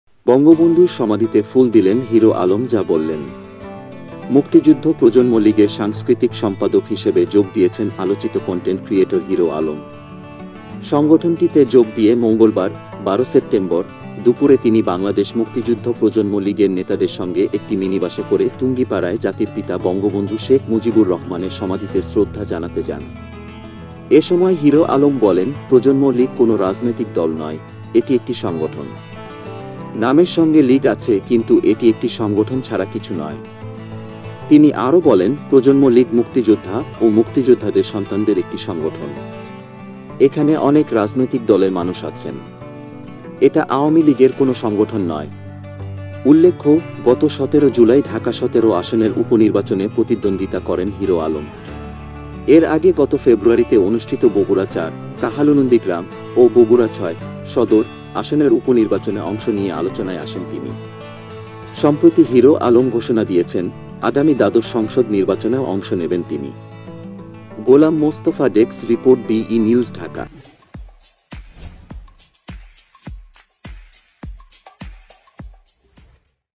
বঙ্গবন্ধুর সমাধিতে ফুল দিয়ে যা বললেন হিরো আলম | Hero Alom | BE NEWS
বঙ্গবন্ধুর সমাধিতে ফুল দিয়ে যা বললেন হিরো আলম | Hero Alom | BE NEWS is the Bangladeshi online News  Channel, Where we makes all the news contents and program materials with the own team or employees.